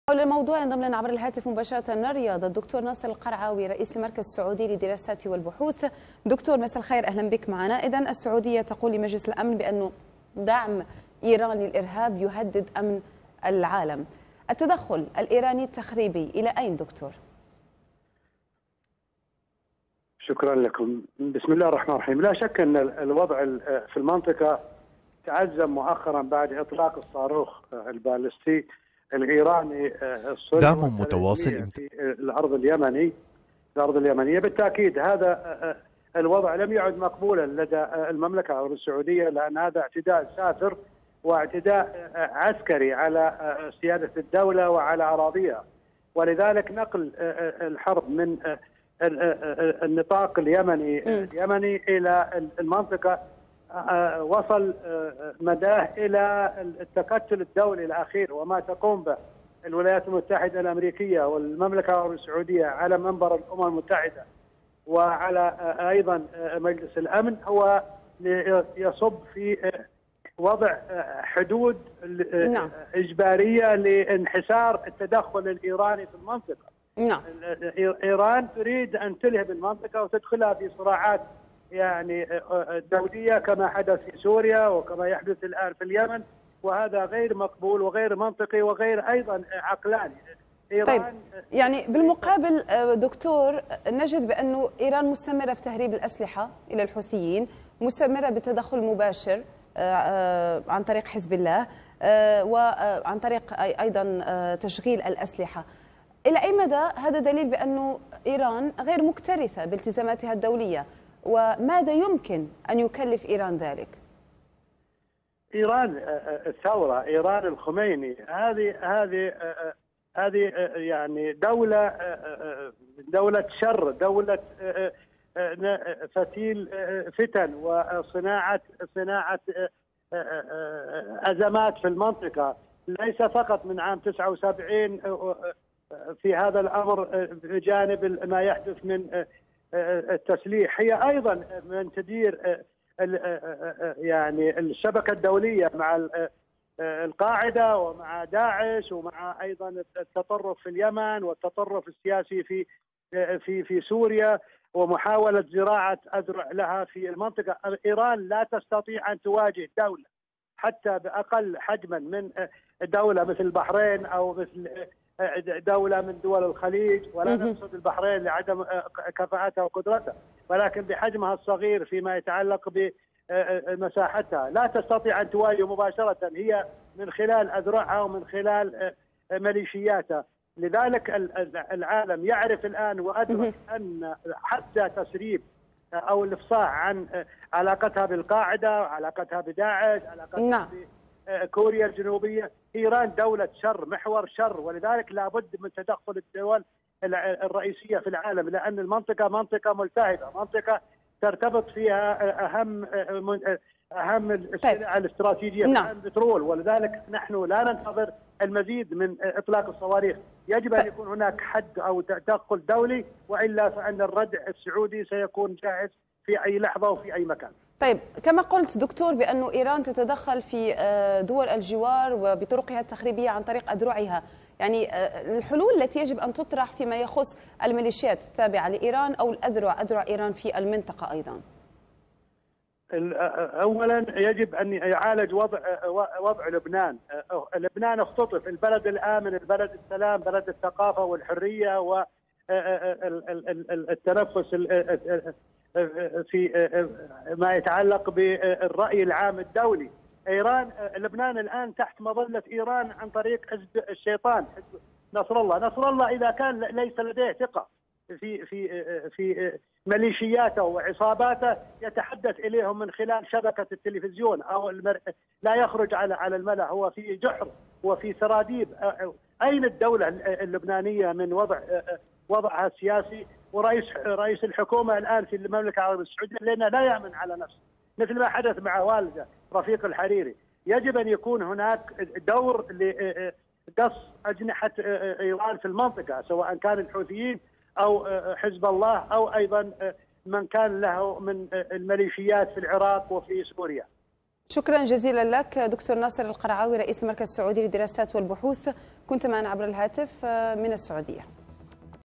تفاصيل عن دعم ايران للحوثيين في اليمن
معنا عبر الهاتف من الرياض  د.ناصر القرعاوي / رئيس المركز السعودي للدراسات والبحوث